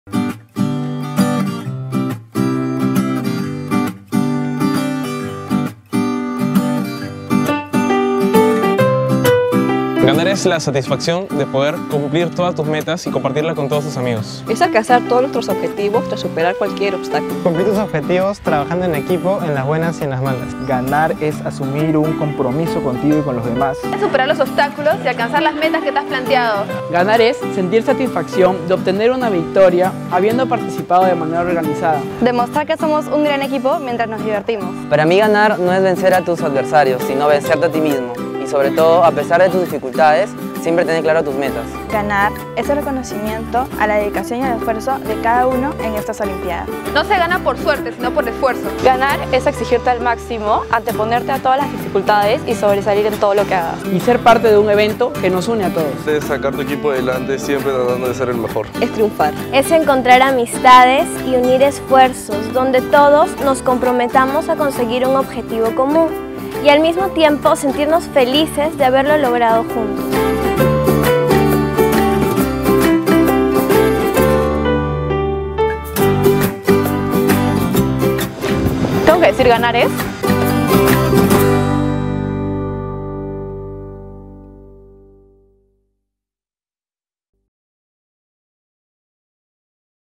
Ganar - Olimpiadas Ramón Mugica
El sábado 18 de octubre se inaugurará las Olimpiadas Ramón Mugica en ambos campus de la Universidad de Piura. Los invitamos a unirse y ser parte del acontecimiento.

Se dará un premio a la mejor fotografía de las olimpiadas, para participar comparte tus fotos a través de OlimpiadasUDEP.